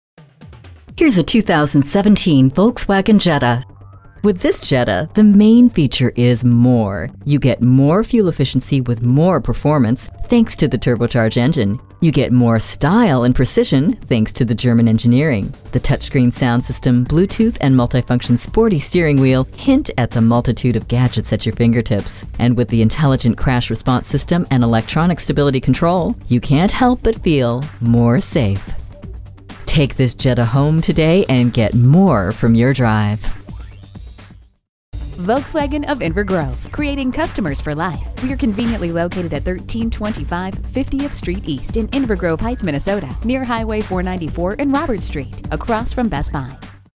New 2017 Volkswagen Jetta Saint Paul MN Minneapolis, MN #82644 - SOLD
Year: 2017
Make: Volkswagen
Model: Jetta
Trim: 1.8T SEL
Engine: I4
Transmission: 6-Speed Automatic with Tiptronic
Color: Black
Interior: TITAN BLACK-QR
Stock #: 82644
VIN: 3VWL17AJ2HM373122
TITAN BLACK-QR Artificial Leather. Turbo! Nav! How do you beat the price at the pump? Just try this this gas-saving 2017 Volkswagen Jetta on for size, that's how. This car is an amazingly fuel efficient car. No longer will you break the bank when you pull up to the gas pump. $3500 offer ends 8/31

Address:
1325 50th Street East